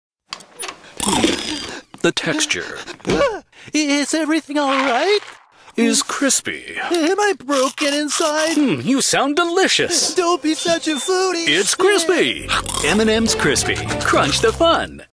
M&M'S Crispy, CRUNCH THE FUN!
Why is Orange so nervous?
What is broken inside?😲
Come find out and enjoy the crunch and fun with M&M’S Crispy!😋

Don't forget to join the M&M'S Screentime Top Master to get prizes!